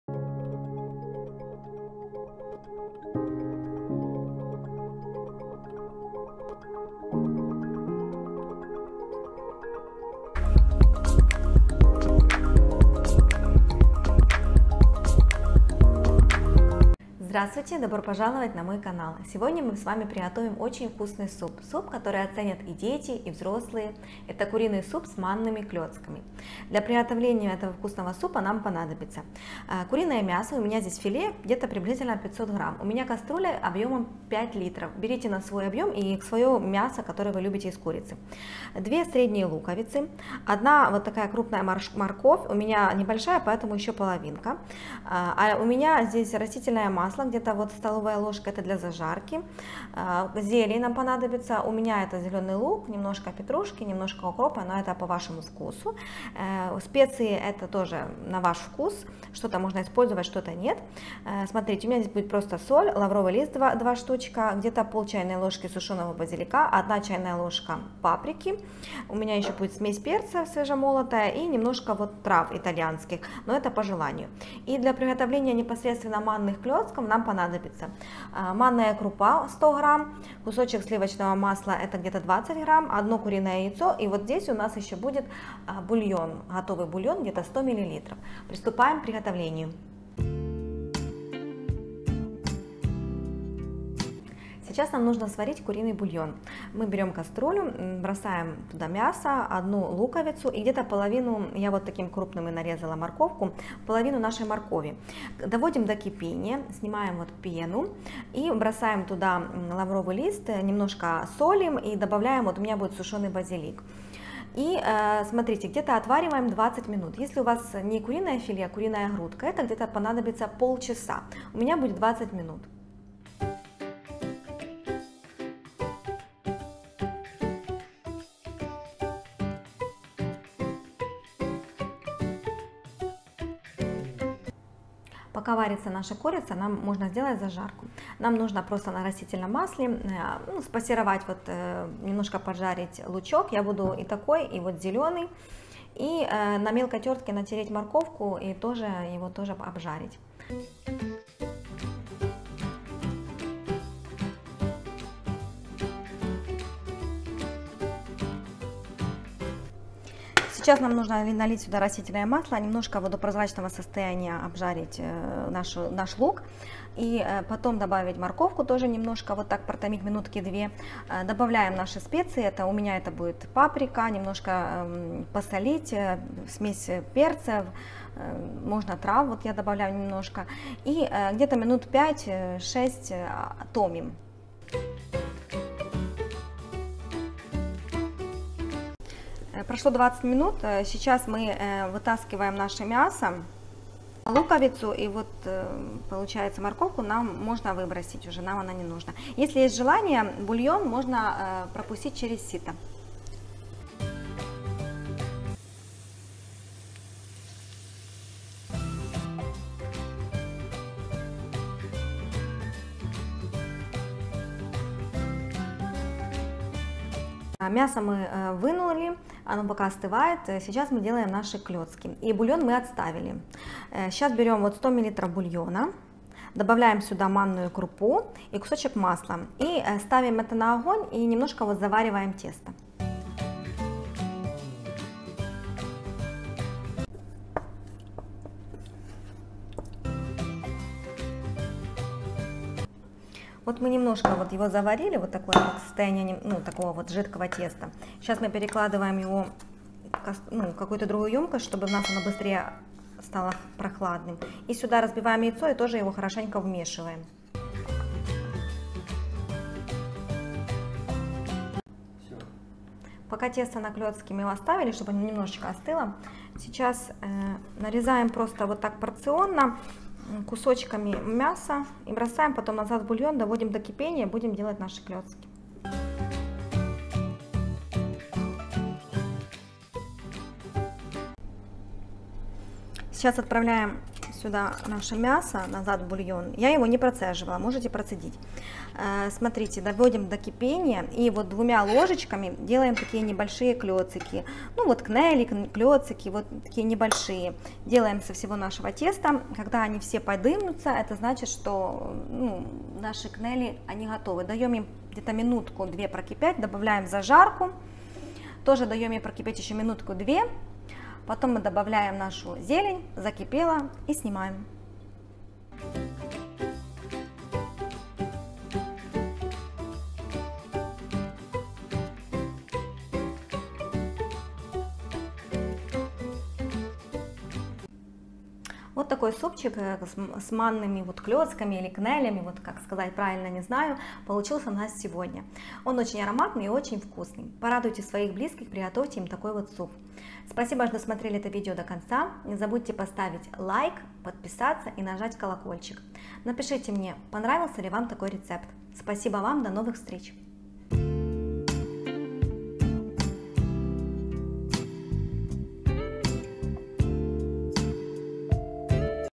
Такой Суп съедается Подчистую! С манными Клецками! Лучший Суп на каждый день!
Здравствуйте!

Сегодня мы с Вами приготовим вкусный и ароматный куриный суп с манными клецками. Этот суп оценят и дети, и взрослые!!!

Давайте приготовим эту вкусноту...
Я буду готовит в кастрюле объёмом 5 литров, но суп у меня будет не до самого верха, поэтому спокойно берите кастрюлю на 3,5-4 литра

Ингредиенты:

Курица (филе, грудка или просто мясо на кости) - по вкусу (у меня 500 грамм)
Лук репчатый - 2 средних
Морковь - 1 крупная
Лавровый лист - 2 шт
Соль, свежемолотый перец - по вкусу
Другие специи - по вкусу и желанию (у меня: 1 чайная ложка паприки, 1/2 чайной ложки сушеного базилика, свежемолотые сухие италийские травы - щепотка)
Зелень (укроп+зелёный лук) - по вкусу и желанию
Растительное масло - 1-2 столовые ложки

Клецки:
Манная крупа - 100 грамм
Куриный бульон - 100 мл
Соль - щепотка
Спасибо за Ваши просмотры!!!

Chicken soup with semolina dumplings!

I will cook in a 5 liter saucepan, but I will not have soup to the very top, so calmly take a 3.5-4 liter pan

Ingredients:

Chicken (fillet, breast or just meat on the bone) - to taste (I have 500 grams)
Carrots - 1 large
Bay leaf - 2 pieces
Salt, freshly ground pepper - to taste
Other spices - to taste and desire (I have: 1 tsp of paprika, 1/2 tsp of dried basil, freshly ground dry Italian herbs - a pinch)
Greens (dill + green onions) - to taste and desire

Dumplings:
Semolina - 100 grams
Chicken broth - 100 ml
Butter - 20 grams
Chicken egg - 1 pc

Thanks for your views !!!